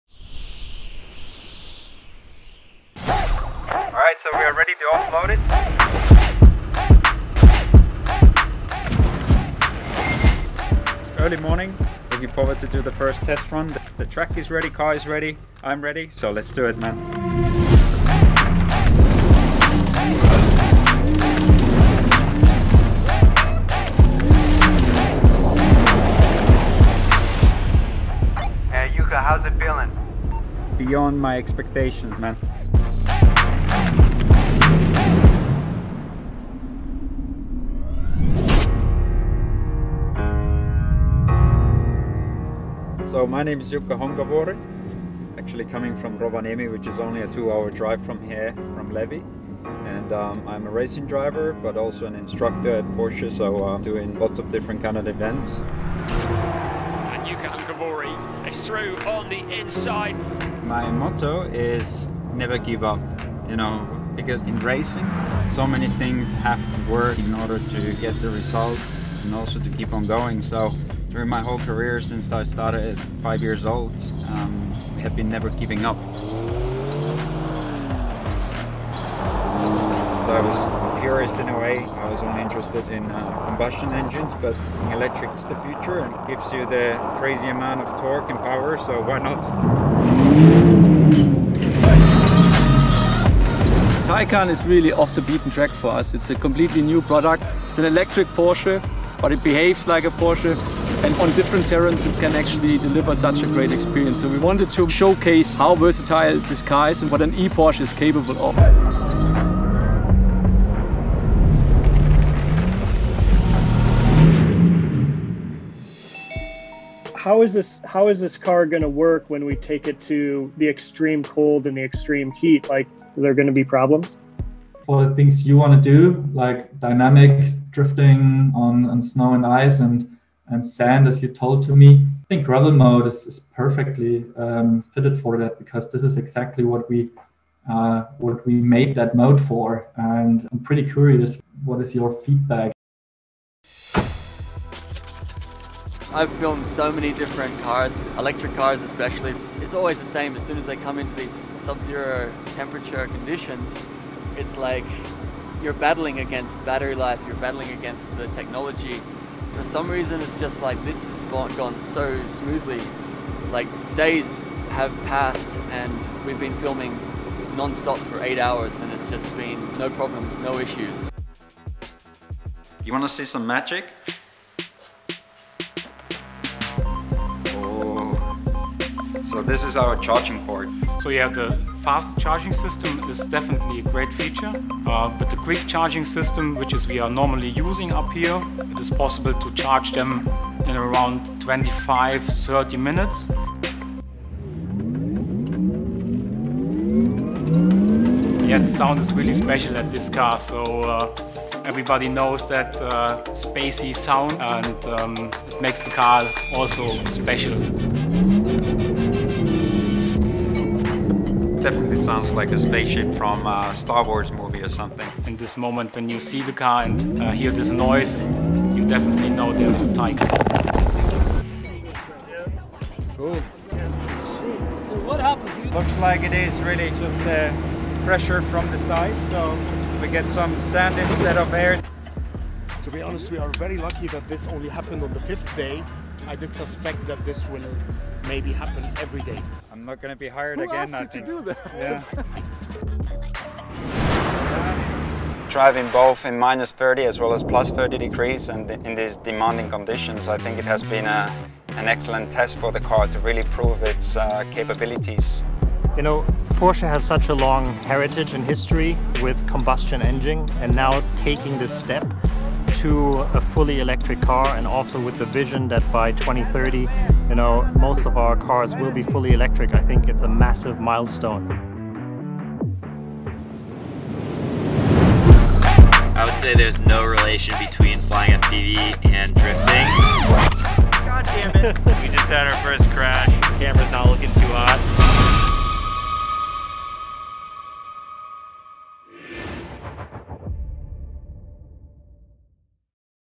Drive2Extremes: Behind The Scenes. Chapter 2: The Driving Force
Man and machine in perfect harmony. Meet the driving force behind Drive2Extremes. The all-electric Taycan Cross Turismo and the man behind the wheel – Jukka Honkavuori. In this episode, we get up close and personal and discover how car and driver came together as one to drift across the extremes seamlessly.